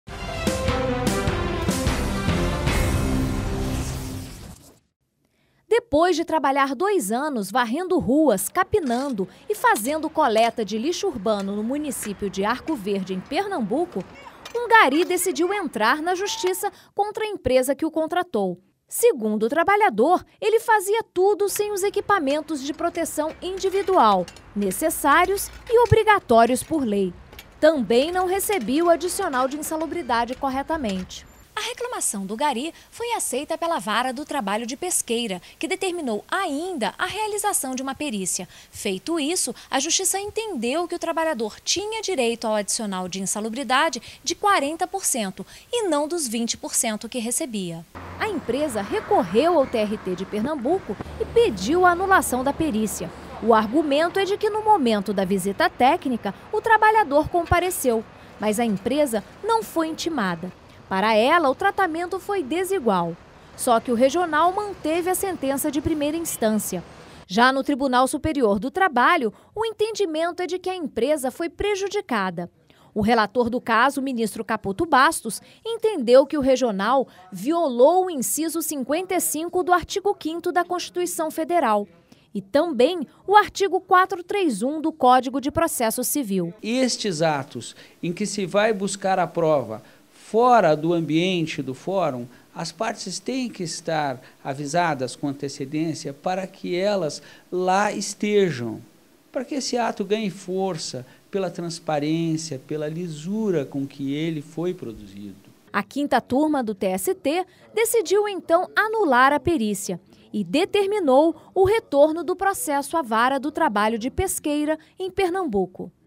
Turma considera nula perícia efetuada sem intimação da empregadora
Tribunal Superior do Trabalho considera nula perícia feita sem intimação da empregadora. O caso aconteceu em Pernambuco. Veja mais detalhes na reportagem de Christiane Meireles.